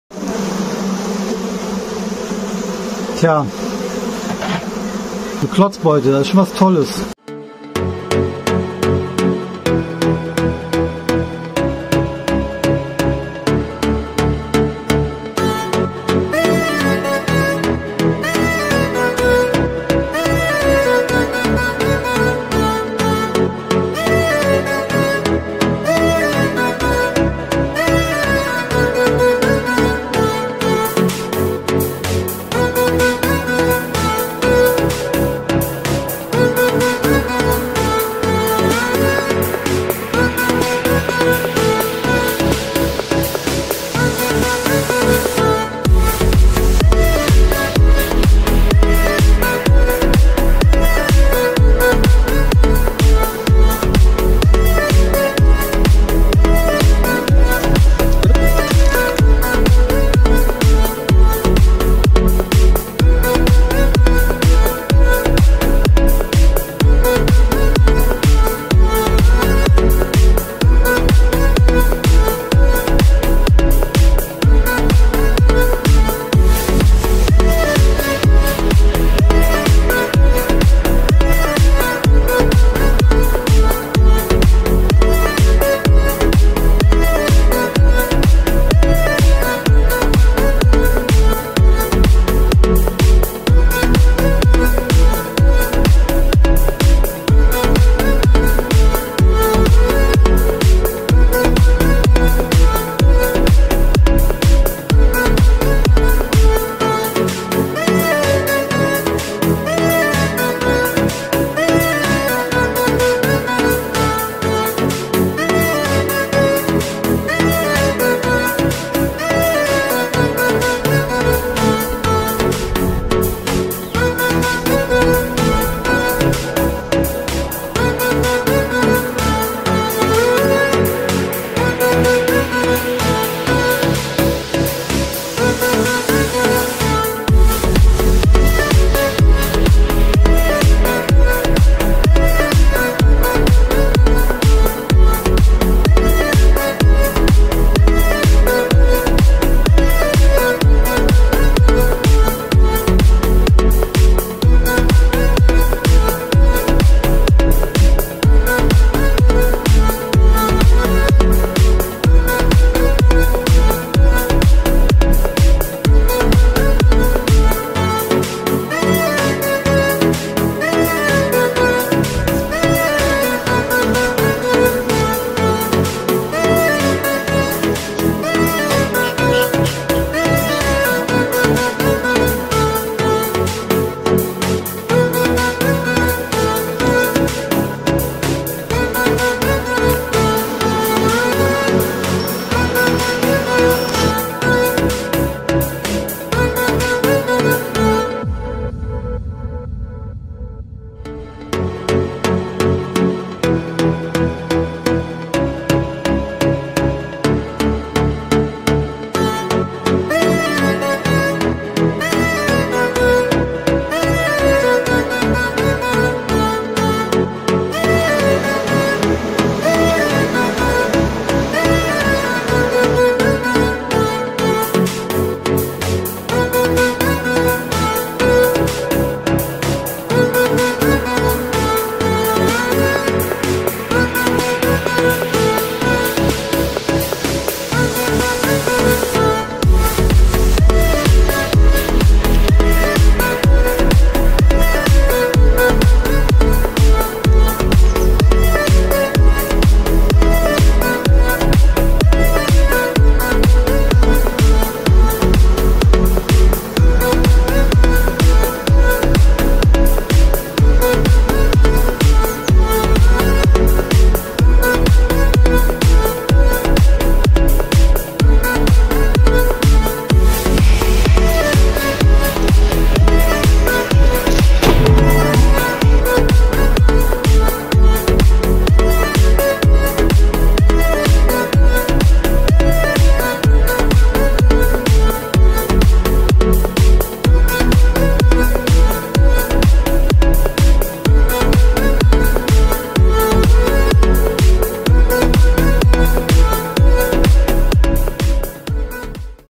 Klotzbeute auflösen
Klotzbeuten sind eine alte Art Honigbienen zu halten. Auch heute noch haben viele Imker ihren Spaß daran.

An dem Betrieb muss jedoch einiges beachtet werden. Varroavorsorge und die Möglichkeit gegen Krankheiten zu behandeln sind vorgeschrieben.

Dieser ausgehöhlten Baumstamm wurde von einem Imker nach dem Verkauf seines Hauses einfach im Garten stehen gelassen.

Auf Nachfrage "erneuerte" sich das Volk immer wieder. Das bedeutete nichts anderes als, dass die Bienen immer wieder starben und ein neuer Schwarm Quartier
 bezogen hat. So auch dieses Jahr.

Auf den Waben war nur altes Futter und keine Brut zu finden.

Da diese Vorgehensweise Bienenseuchen wie die amerikanische Faulbrut fordert, haben wir den Baumstamm auseinander gebaut und die Bienen in eine andere Kiste umgesiedelt.

Sie werden jetzt fachgerecht versorgt. Um Krankheiten vorzubeugen, würden die Waben direkt am Anschluss ausgeschmolzen und die Rähmchen verbrannt.

Vielen Dank auch an Michael Seidl für die Fotos seiner Klotzbeute.